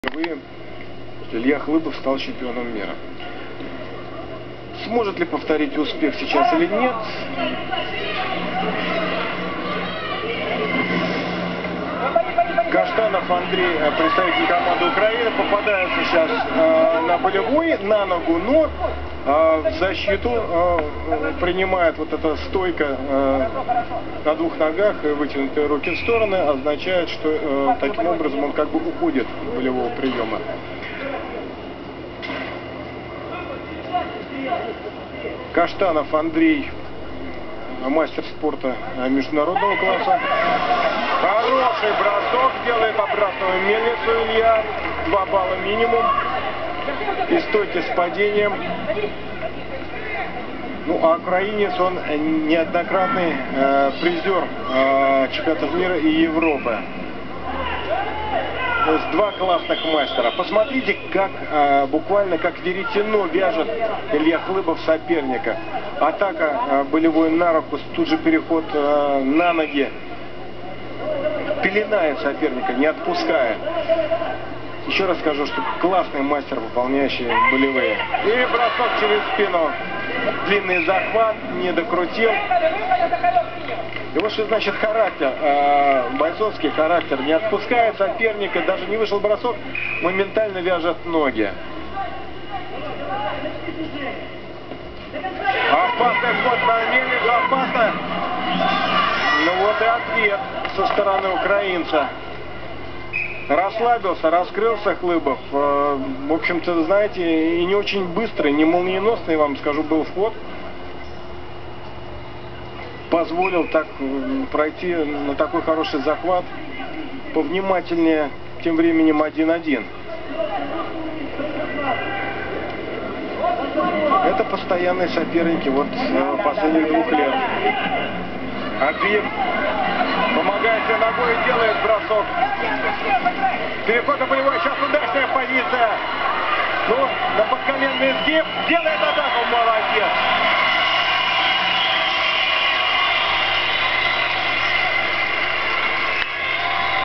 World Sambo Championship 2008: final 62 kg, Khlybov (red, Russia) - Kashtanov (blue, Ukraine)
World Sambo Championship 2008, Saint-Petersburg, Russia: final 62 kg, Khlybov (red, Russia) - Kashtanov (blue, Ukraine)